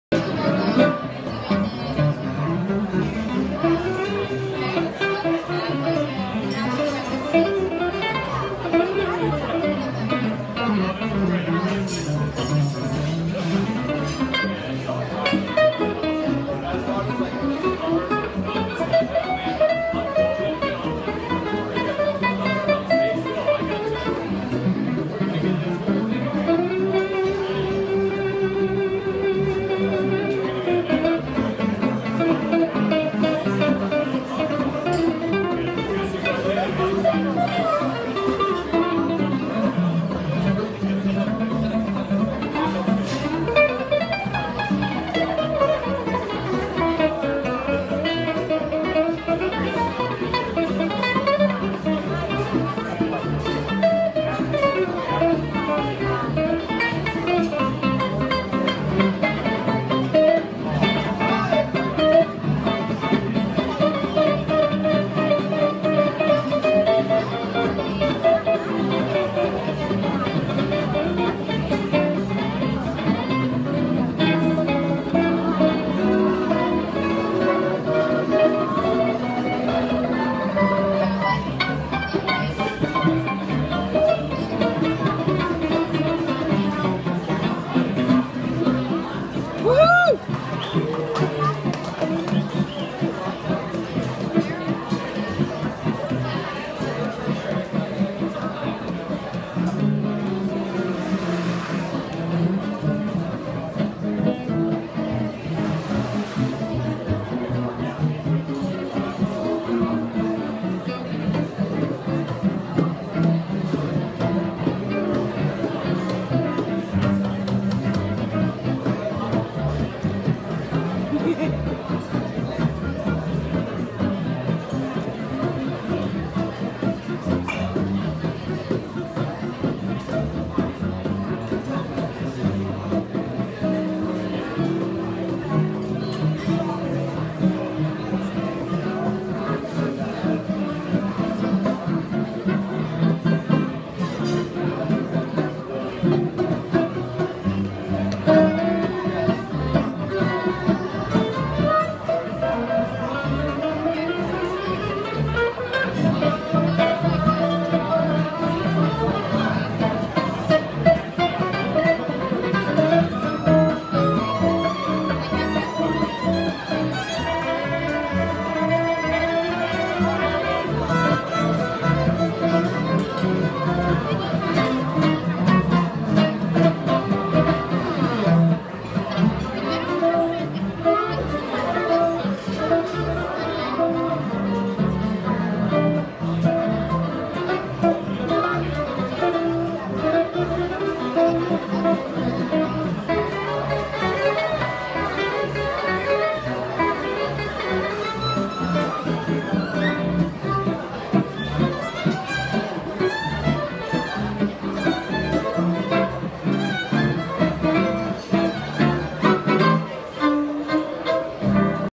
The Gypsy Hombres in Nashville
Some exquisite sounds for brunch this morning in Nashville. Enjoy.